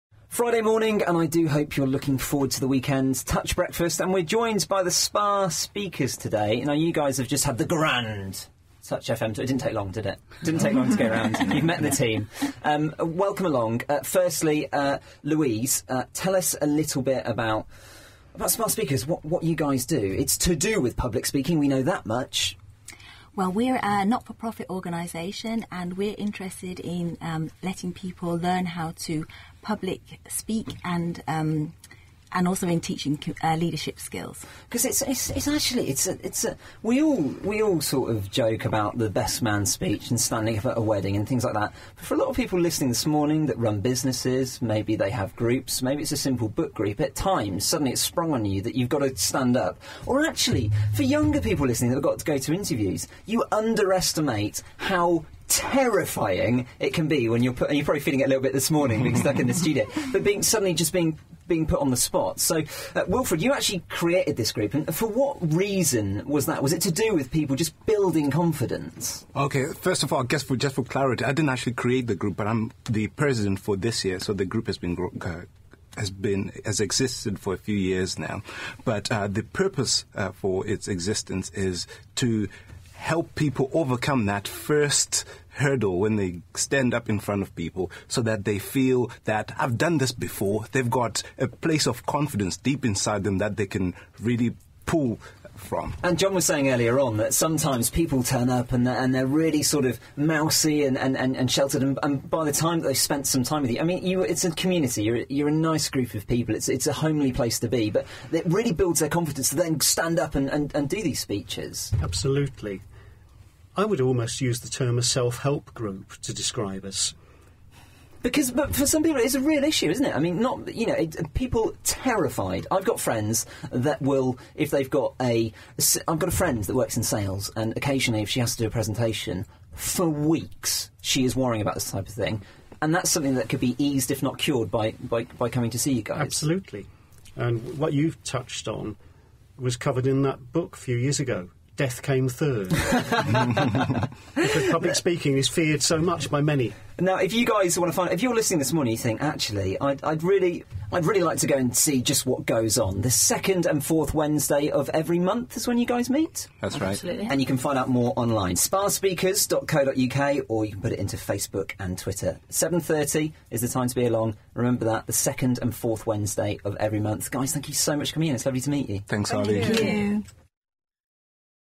Spa Speakers with Ollie Gallant at Touch FM radio
A short interview with DJ Ollie Gallant on 102 Touch FM, when Spa Speakers were invited for a tour of the studios. In an interview at the end Ollie wanted to discuss what Spa Speakers has to offer.

From SpaSpeakers: Carla Alves da Silva, Louise Weickert, Andrea Kingston, Jill Roberts, John Holder and Wilfred Denga